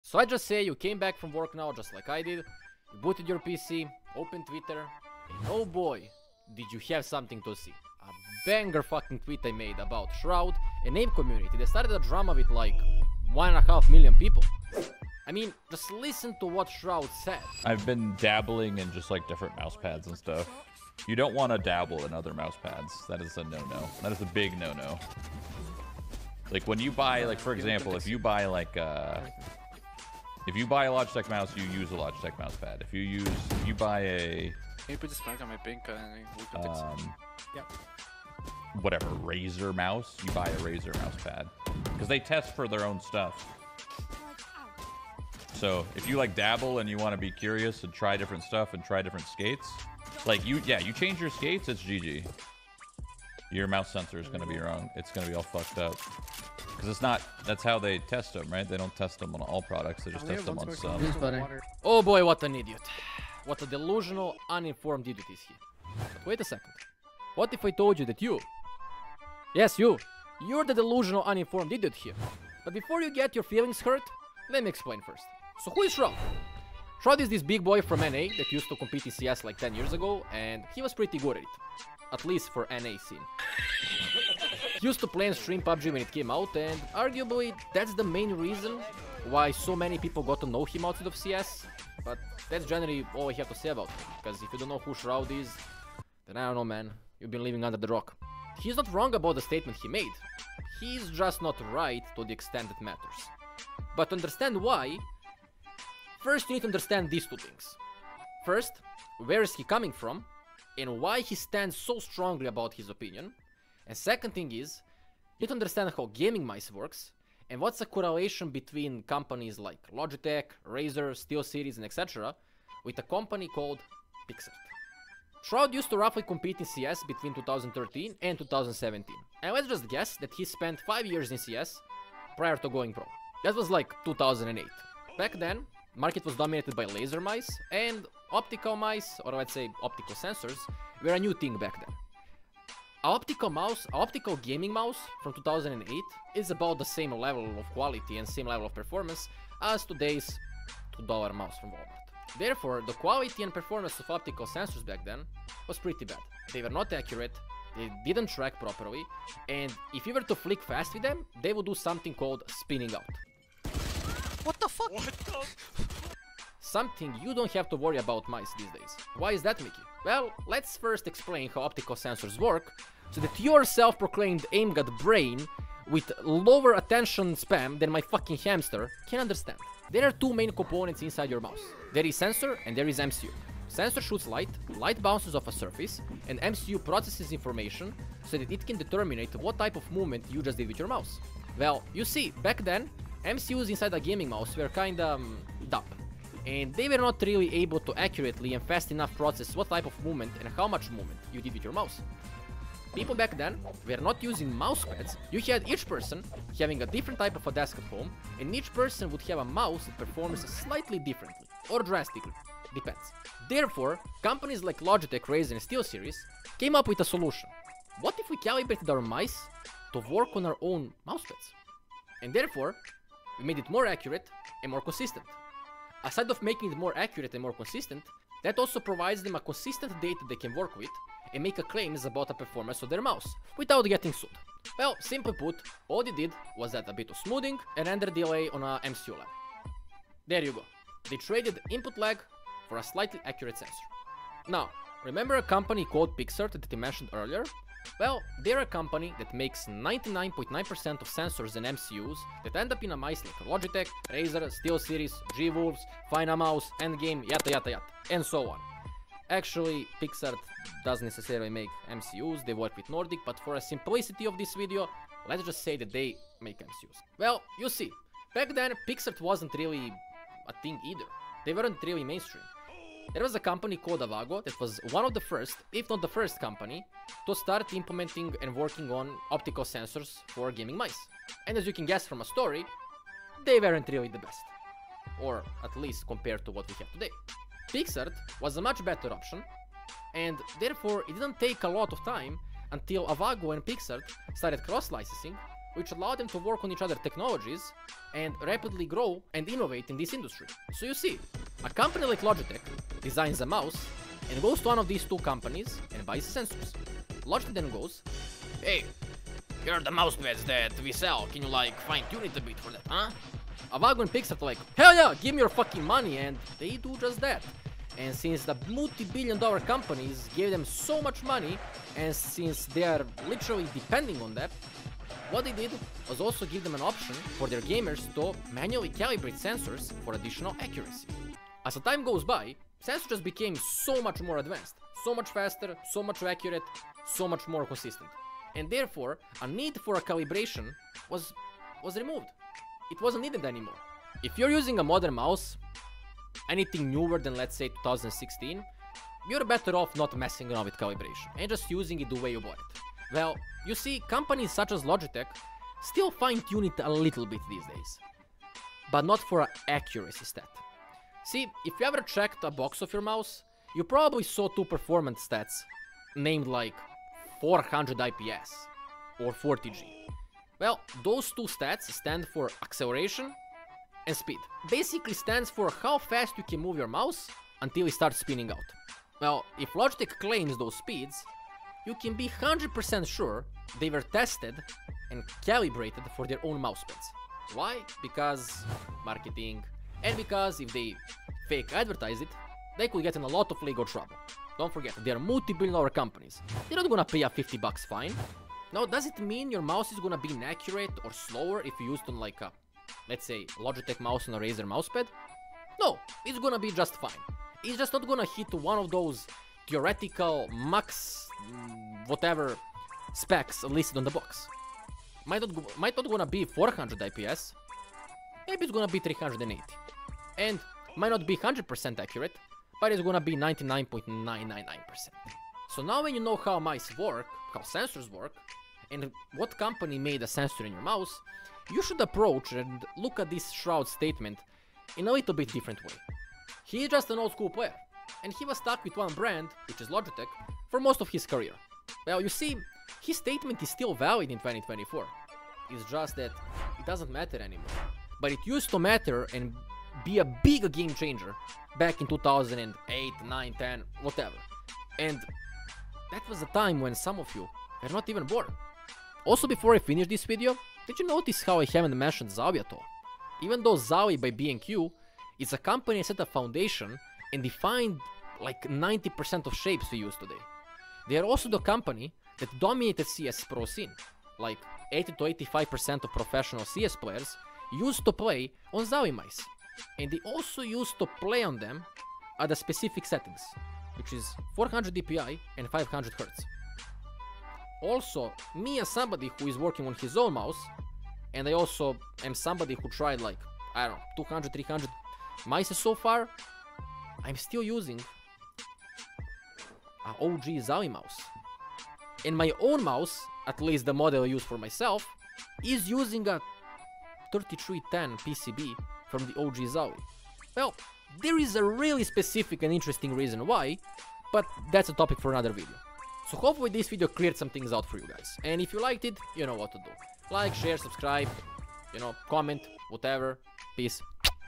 Shroud is OUTDATED... sort of
Video was a bit rushed and before people with actual knowledge about mice and sensors jump on me for the way I explained how sensors work. I know that best way of describing it would be that sensor takes images of a surface to measure texture, height and etc. but for a sake of simplifying it as much as possible I decided to do it this way.

Wanna hear about my own mouse, The Contender?